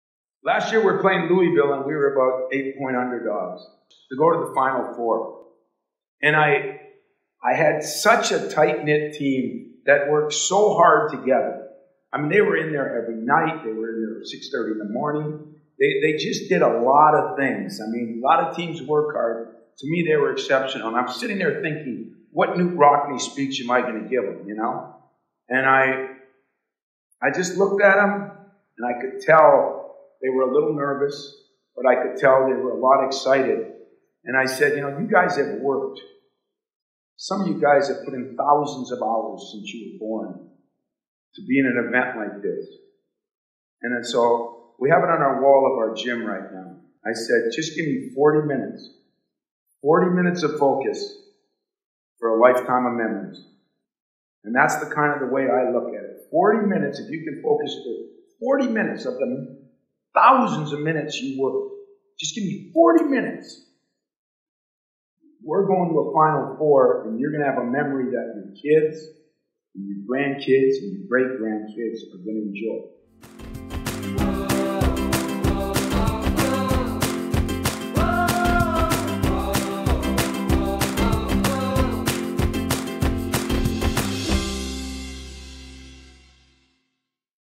PANELS 2015: Tom Izzo details his famous "40 Minutes" pregame speech
During his keynote speech at PANELS 2015, Tom Izzo explains what motivated him to give his now famous "40 Minutes" pregame speech before beating Louisville in the Elite 8 of 2015 NCAA basketball tournament.